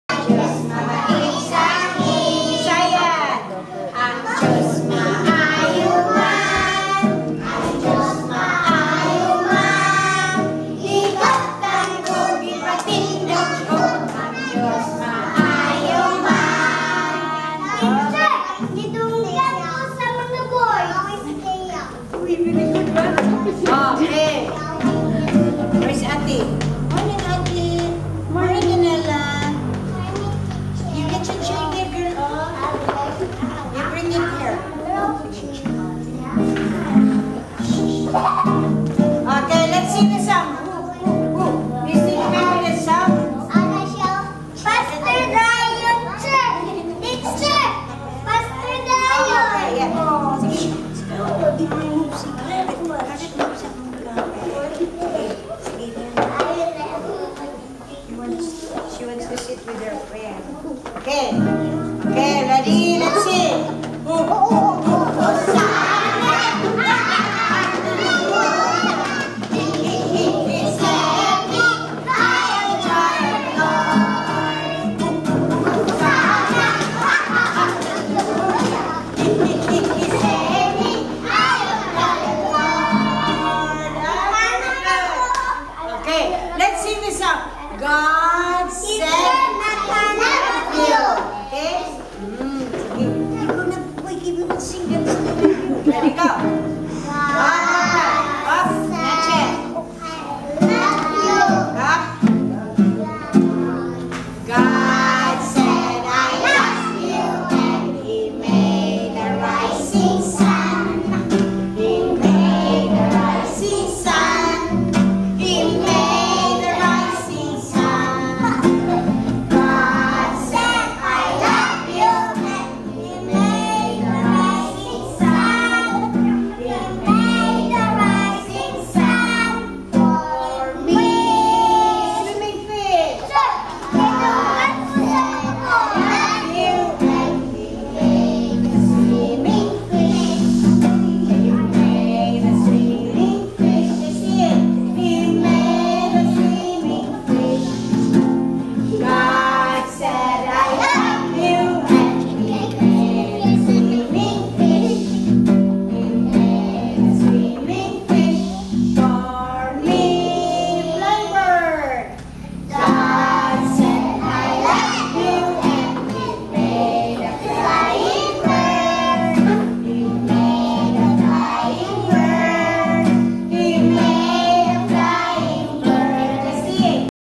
Kindergarteners at Bethany Christian School in the Philippines
Kindergarteners at Bethany Christian School in Talakag Philippines singing.